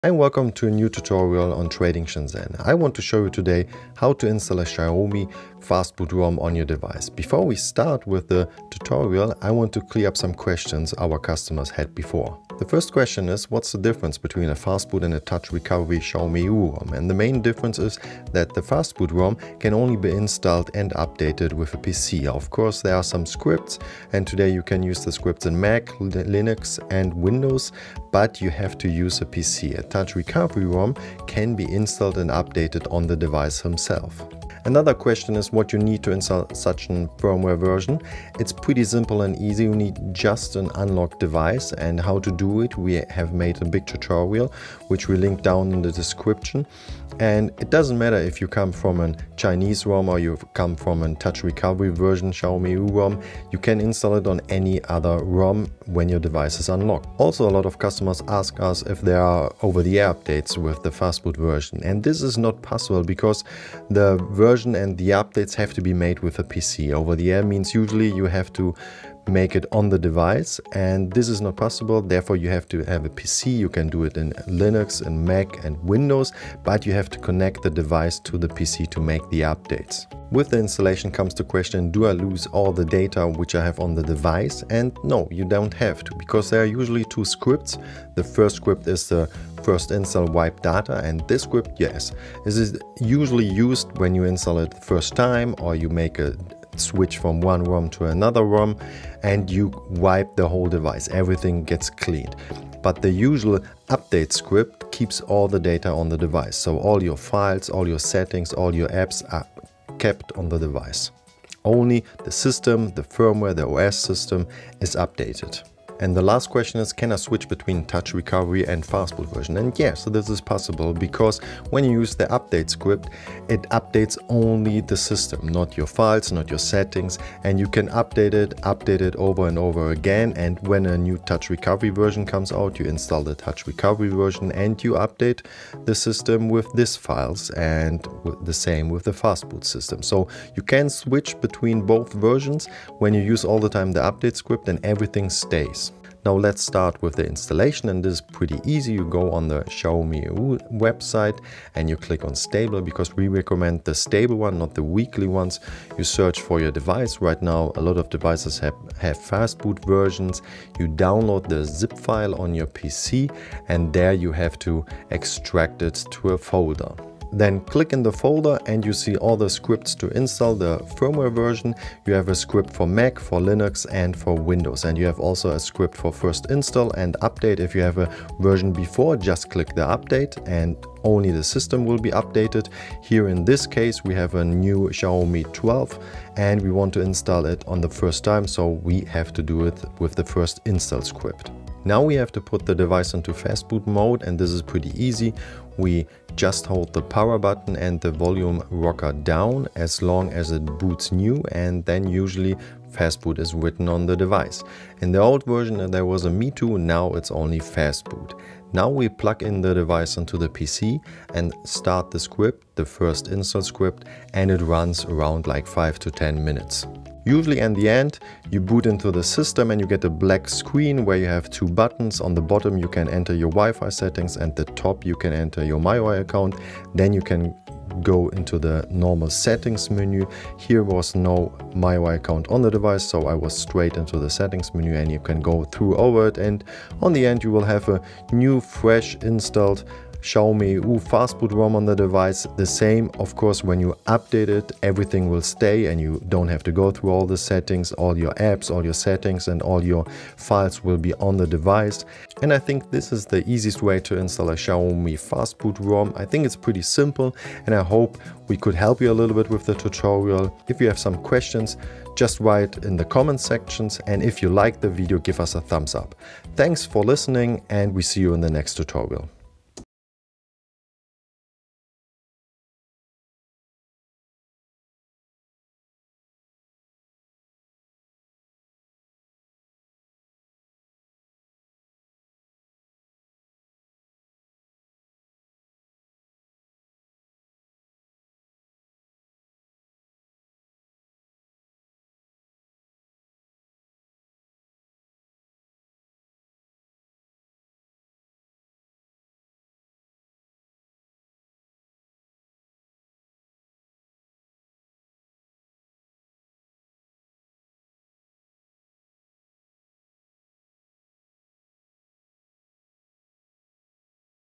Xiaomi.eu Fastboot Rom Flash Tutorial - Questions and Installation - English
IMPORTANT!
This tutorial is still valid, the only thing that changed nowadays is the unlock process. It is way more difficult to unlock a Xiaomi device than it was before. Usually, you need an extra service for it, where we use But it is quite expansive to unlock today.
IMPORTANT!

If you have a current Xiaomi smartphone and want to install a new Xiaomi.eu Rom, then this is often only available as a fastboot version.

This is not so bad and in this tutorial we cover 5 frequently asked questions about a fastboot rom and how to install it.

Problem - Waiting for device
If you face this problem, it is maybe because some drivers are missing. Install and usually it installs with the first install the drivers. If not, klick on settings and drivers install.

CHAPTER SELECTION

0:12 What is the difference between the Roms?
0:34 What you need to install it?
0:57 Are OTA Updates possible?
1:20 Do I loose all my data when updating?
1:55 Can I switch between the Roms?
2:26 Installation of the Fastboot Rom!

-= Trading Shenzhen =-